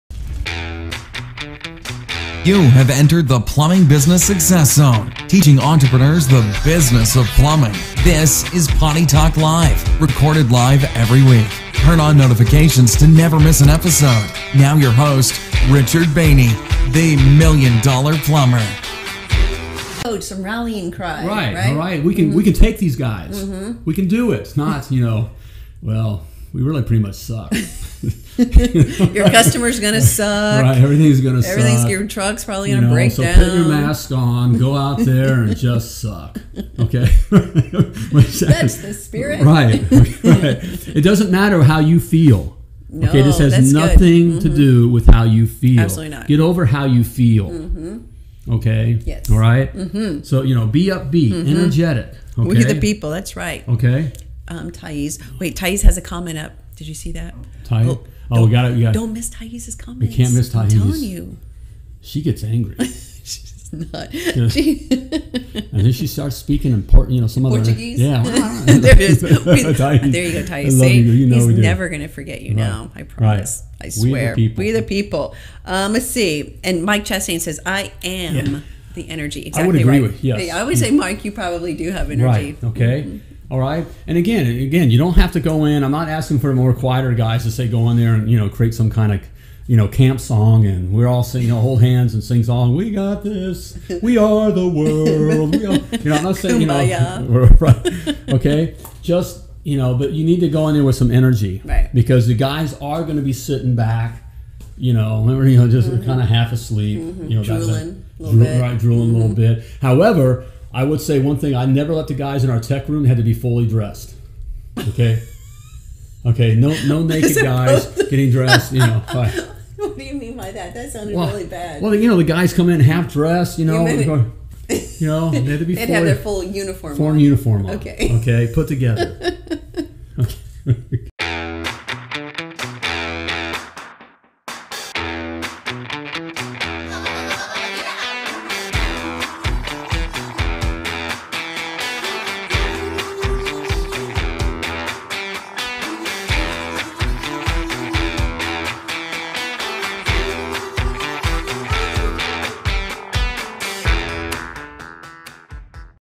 Advice for Running a Meeting With Your Plumbers
Your weekly meeting with your plumbers is one of the most important parts of your week.

It should work like clock work - same day, same time, every week.